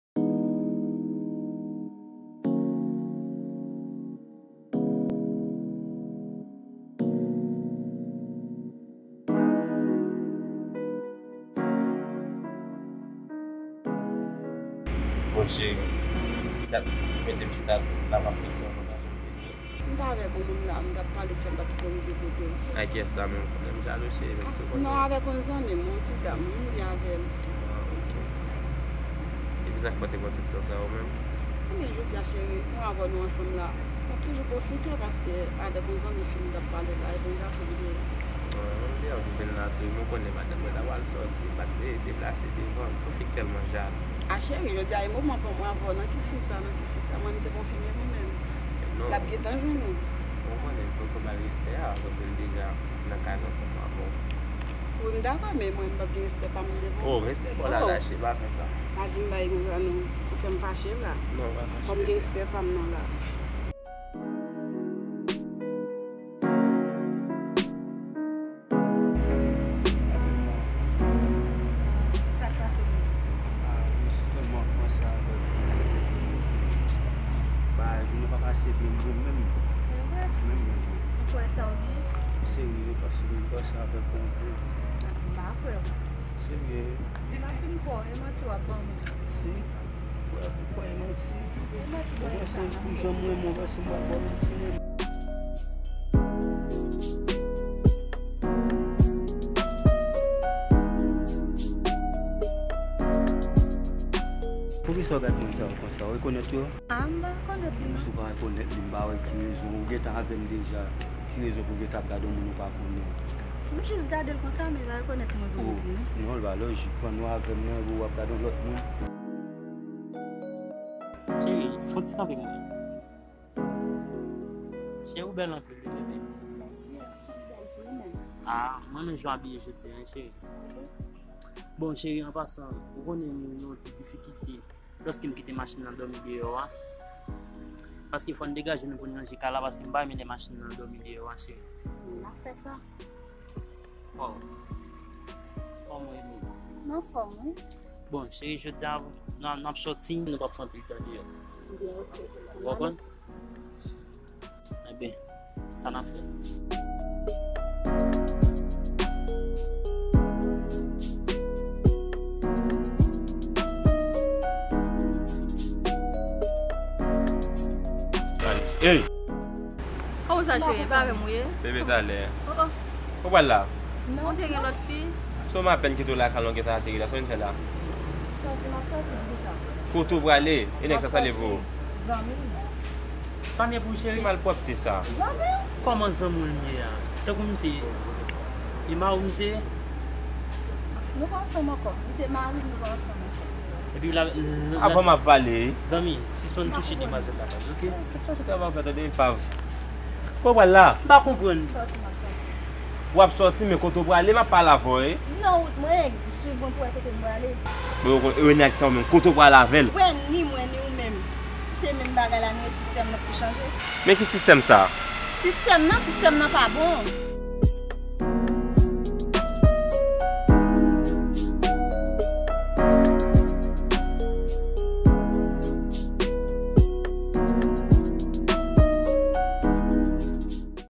Ann Chanje Sistem lan | @Cho SAJ Média | @Smart Production
Nou tout ap pale de sistem lan, pandan ke se nou menm ki sistem lan...

Enjoy ti video pandan nap tann 5e epizod feyton KONPLIKASYON LANMOU an